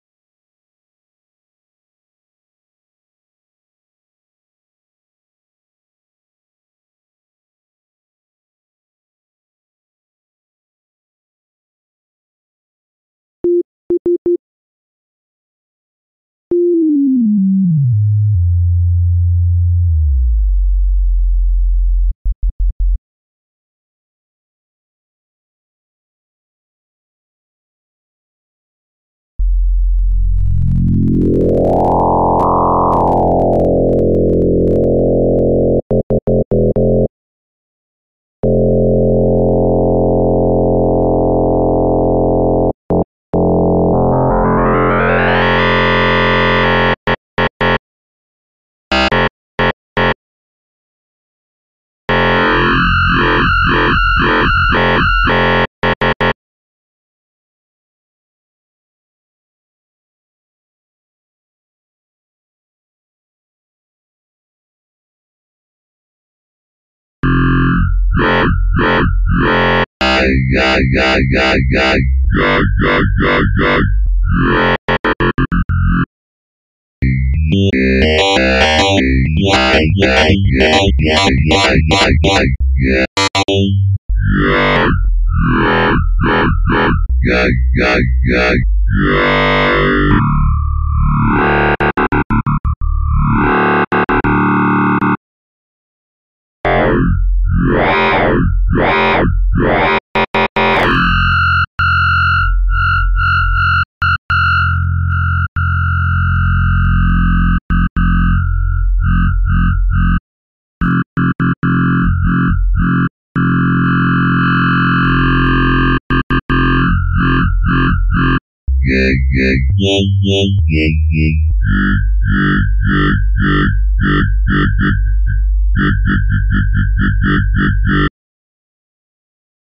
Dubstep Tutorial: Talking Bass with FM Synthesis
A tutorial on how to make a talking bass with Sytrus and FM Synthesis.